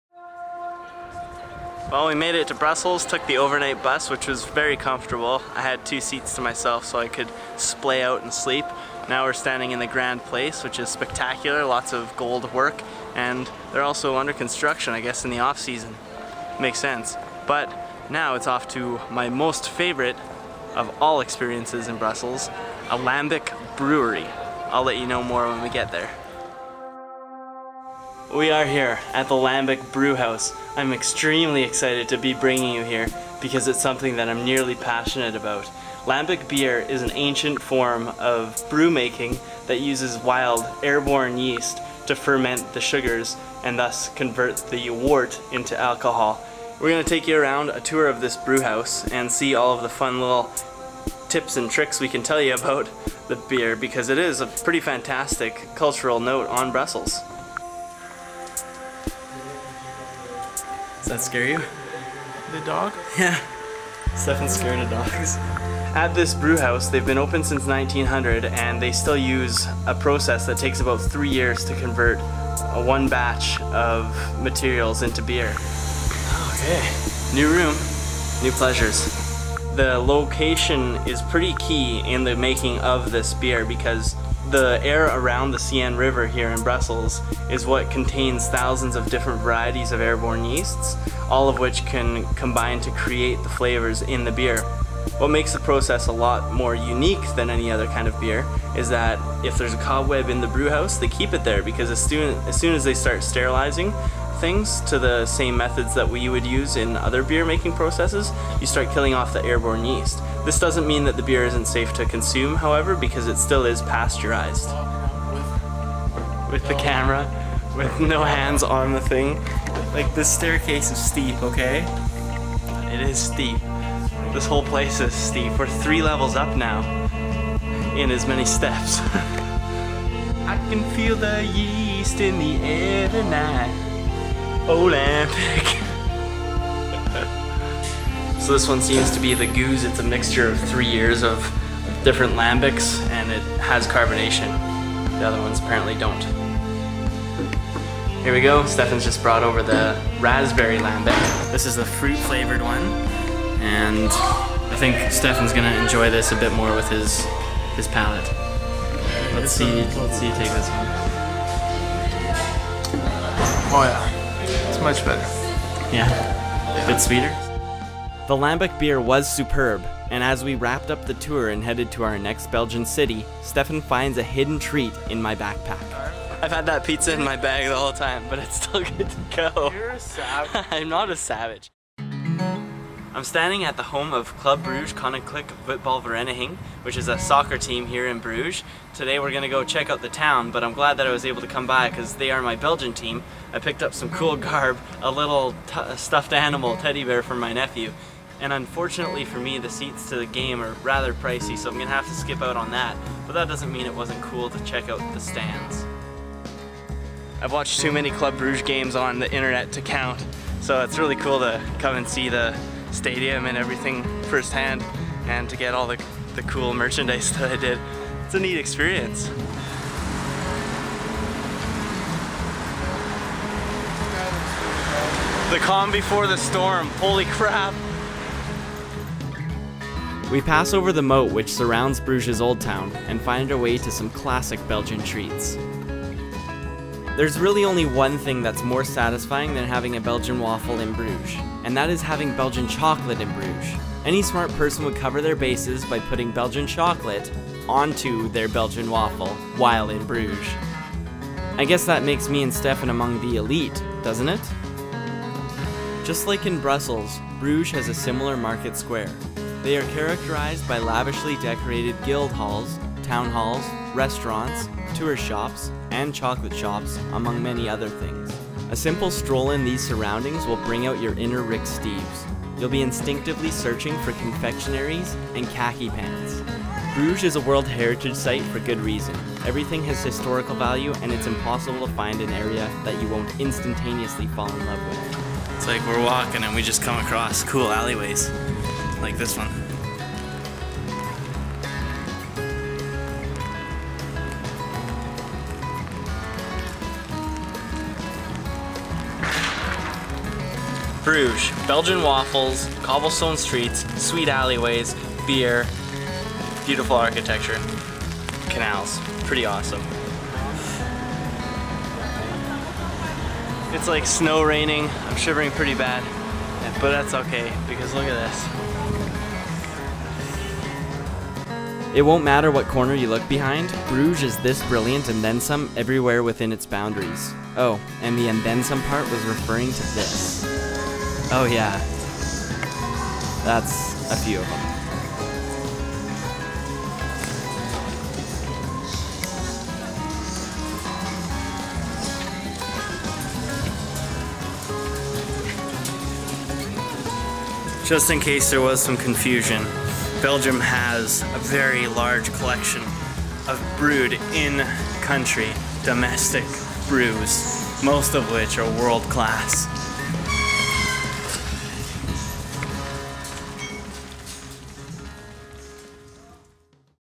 Roam Belgium (Brussels, Lambic, and Club Brugge) Part 1 of 2
Lambic beer is an ancient form of brew-making made famous by the belgians in Brussels.  Jordan takes us on a tour of the last remaining brewery in Brussels to hold true to this ancient style, as one batch of beer can take up to three years to create.  We then head over to Brugge to check out the town and Jordan's favourite Belgian soccer team as it associates with his ancestry.